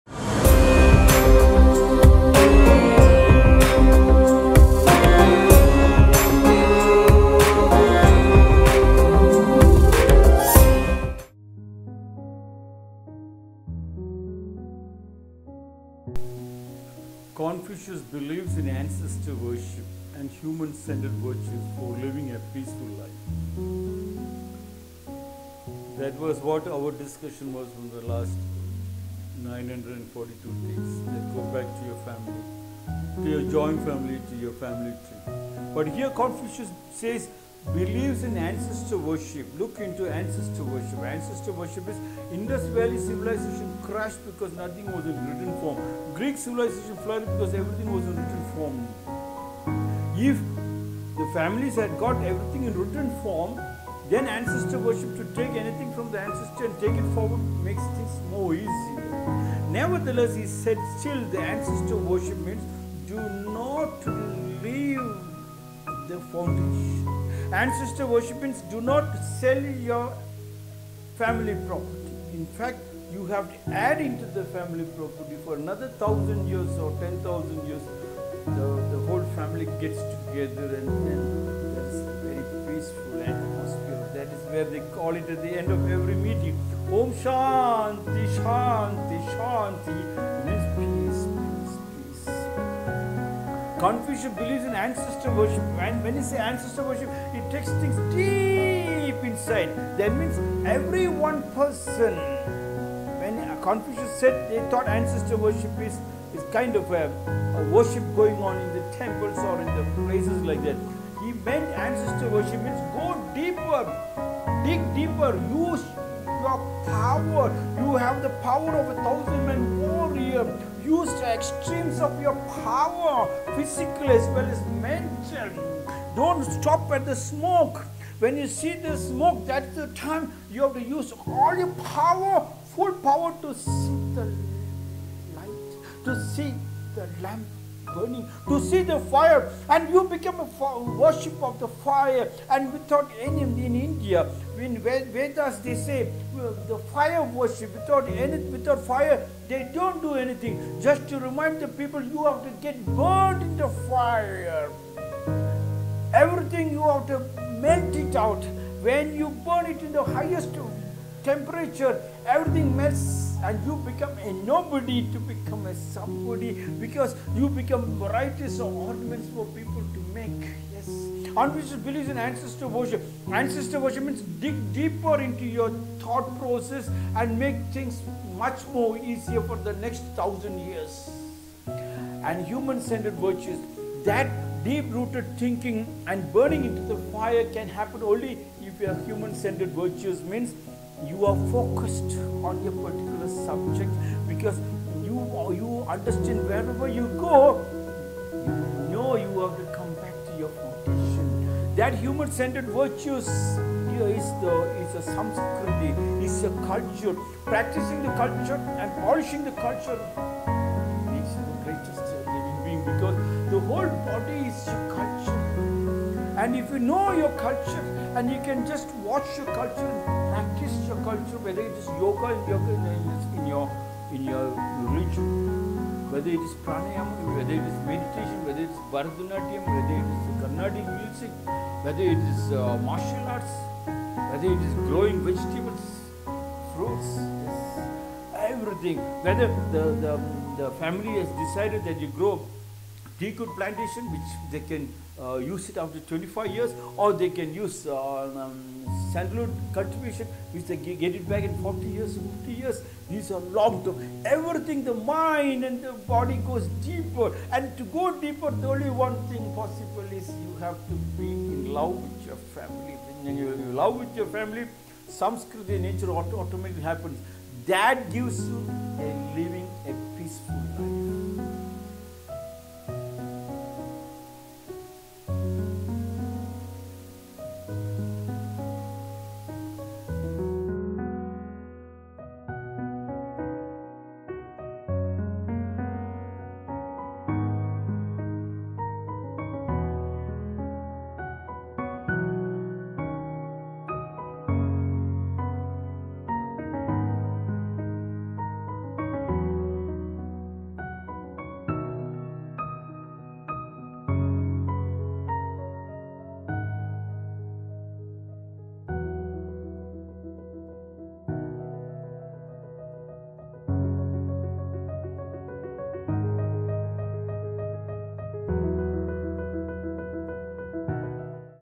Swami Rakumjee, 958th Small talk on Confucius (5) FOR A PEACEFUL LIFE
Swami Rakumjee,

959th Small talk on Confucius (5)

FOR A PEACEFUL LIFE

“Confucius believes in ancestor worship and human centered virtues for living a peaceful life.”